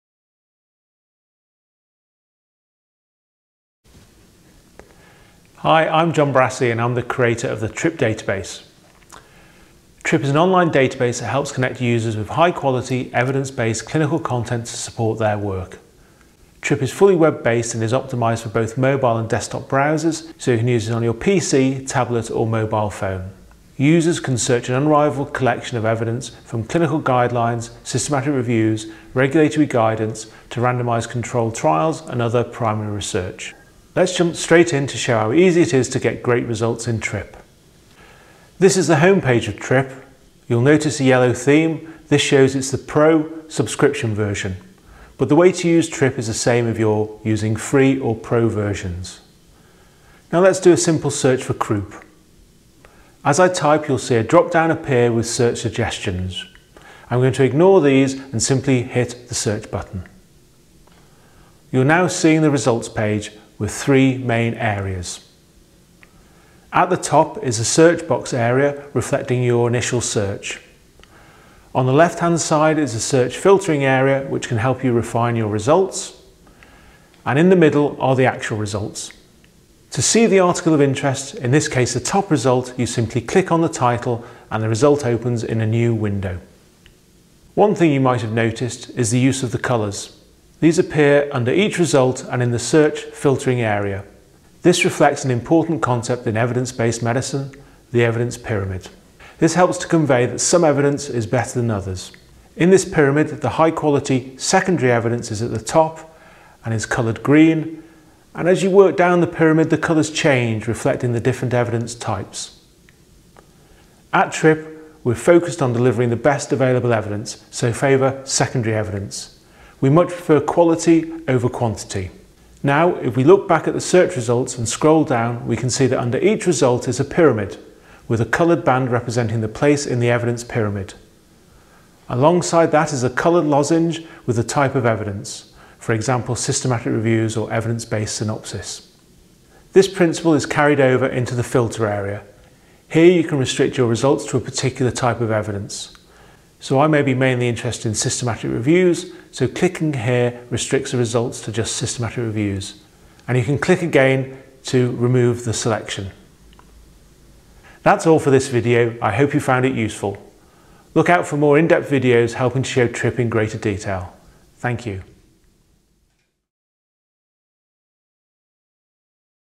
An introduction to the Trip Database
If you're new to Trip and want to understand more of how to use the site, then watch this short video.